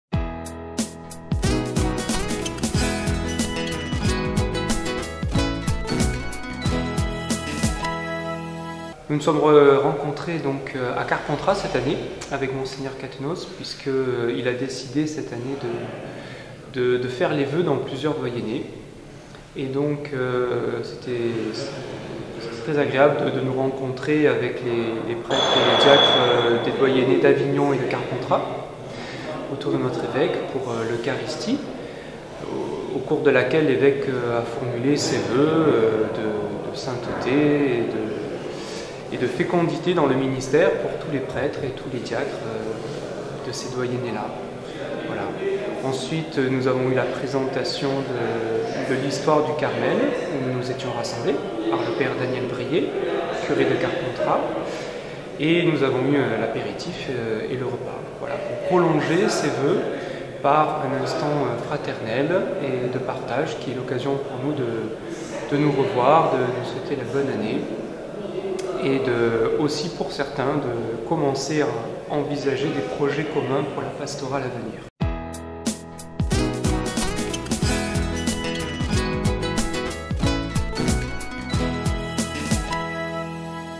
Voeux de l'évêche 2011
Père Yannick Ferraro nous partage les moments vécus à l'occasion des vœux de l'évêche pour l'année 2011 à Carpentras.